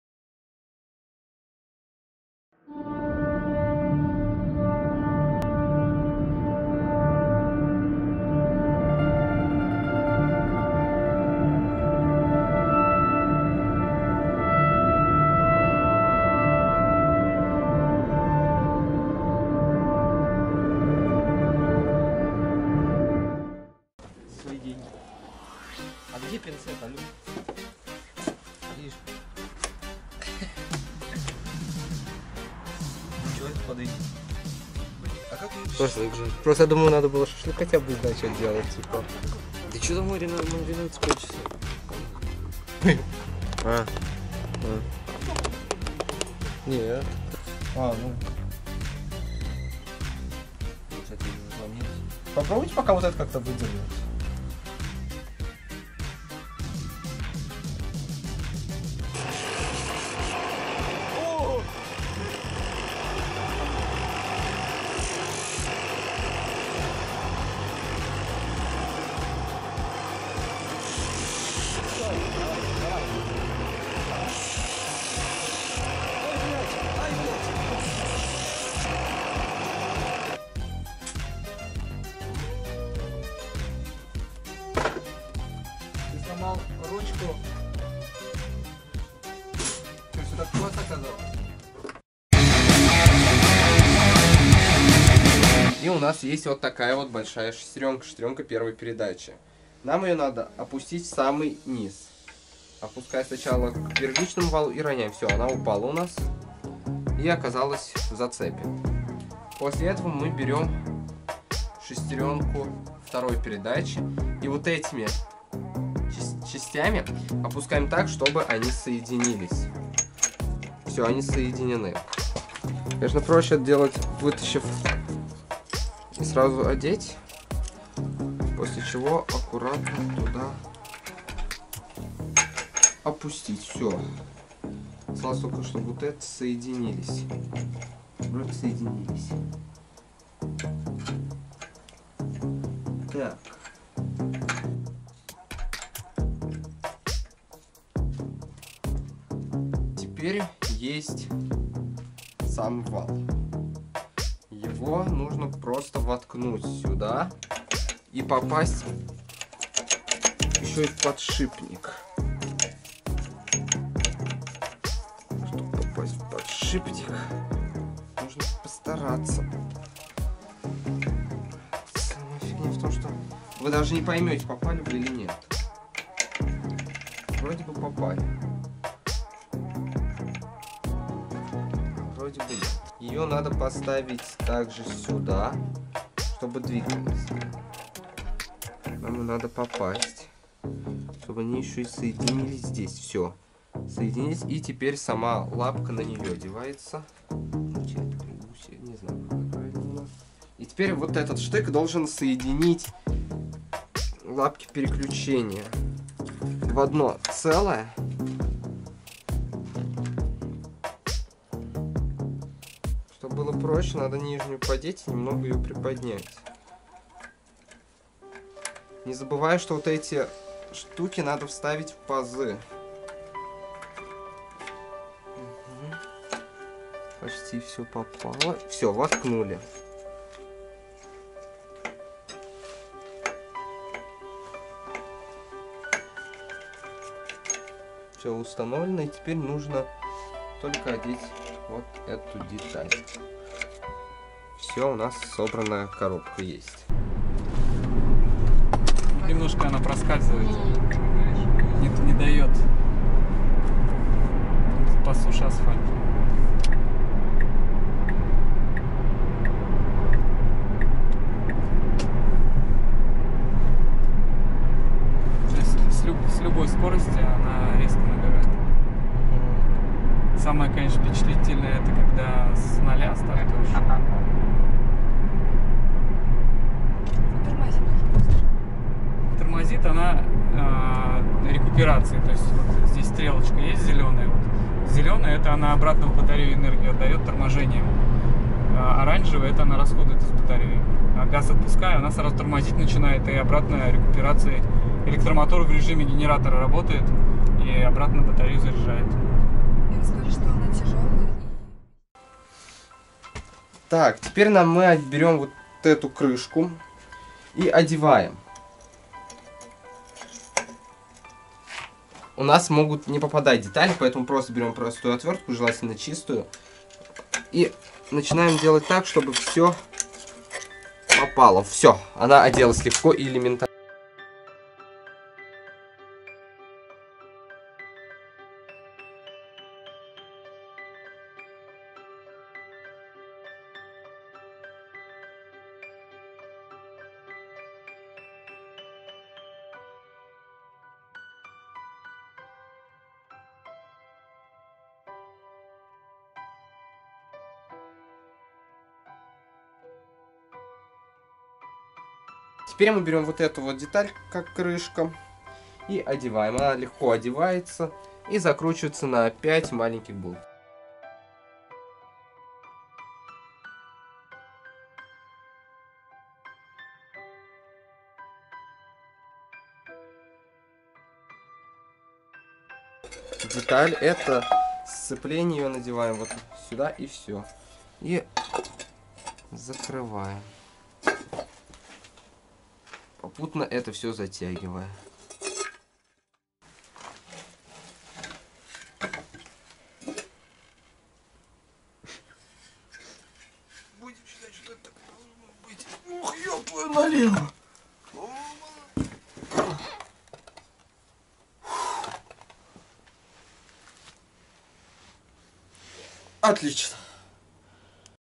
Восстановление Минск #2/Сборка КПП Минск/Ремонт скутера/Пропал газ скутер.
Сегодня я покажу как собрать  КПП Минск. И что делать если пропал газ на скутере.